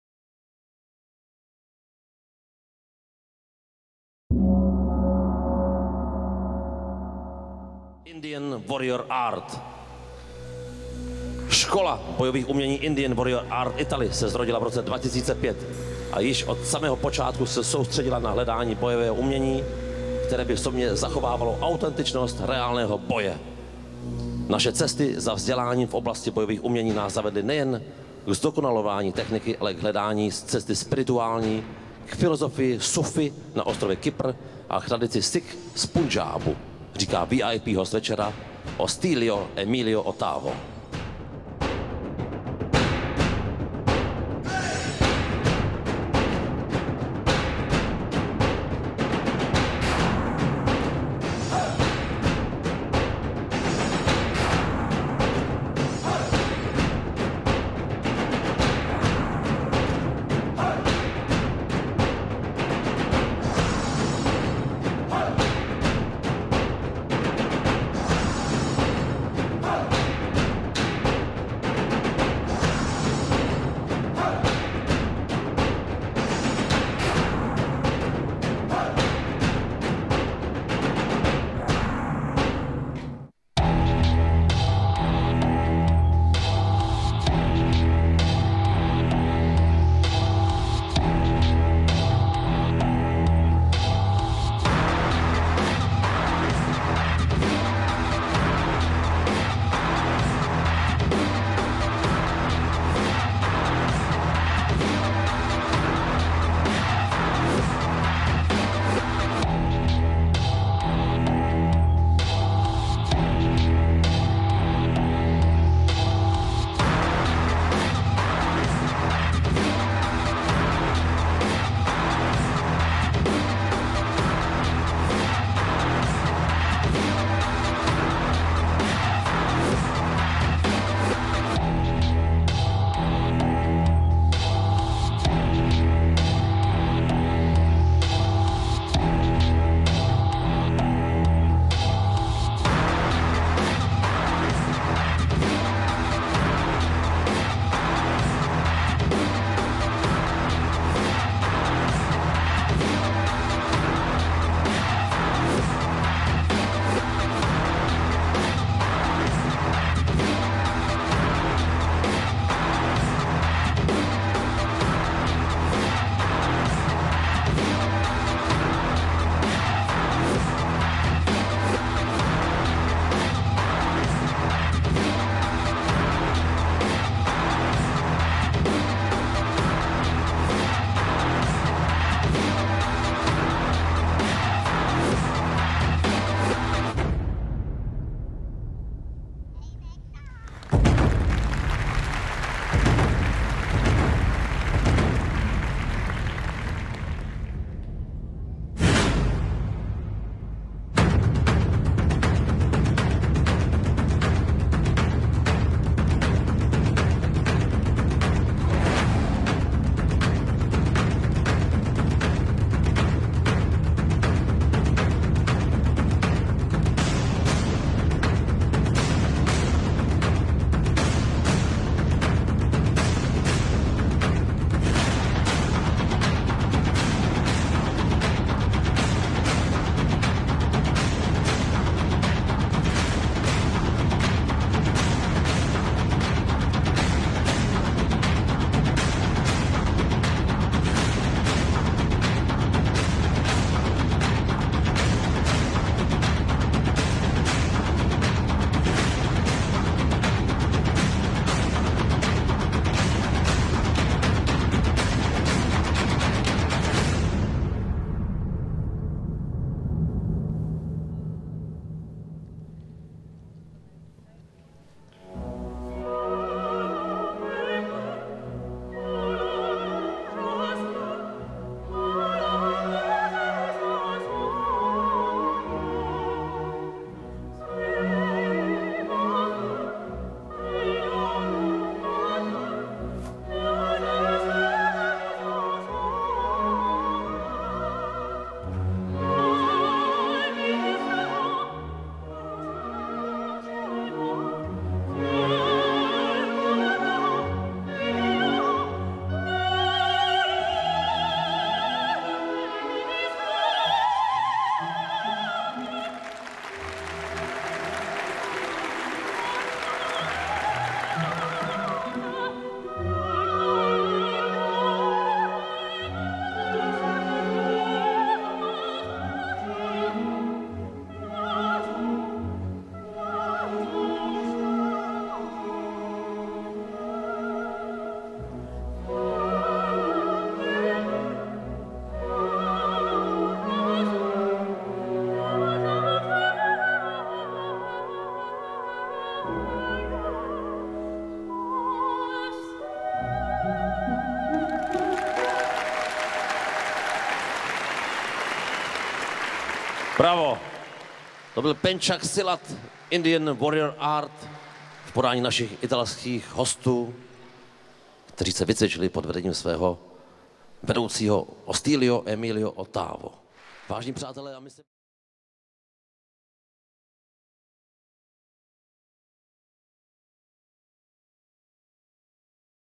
IWA Indian Warrior Art BUDOSHOW 2014
IWA presente al prestigioso evento marziale europeo Budoshow di Brno,Rep.Ceca.